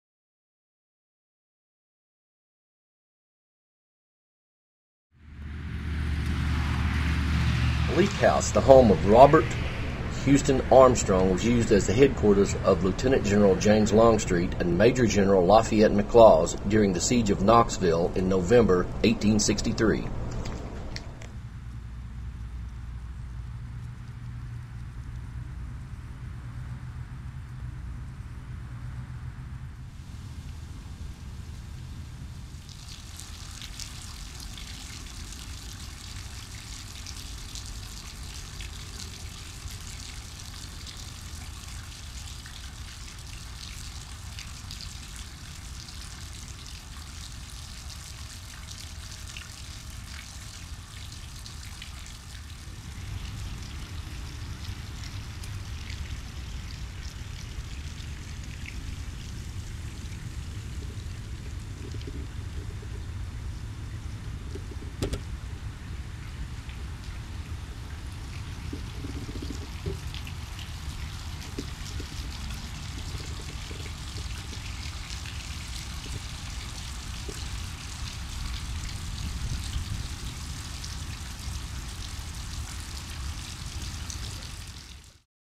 Bleak House (Confederate Memorial Hall)
"Bleak House" the home of Robert Houston Armstrong, was used as the headquarters of Lt. Gen. James Longstreet and Maj. Gen. Lafayette McLaws, C.S.A., during the siege of Knoxville, Nov. 1863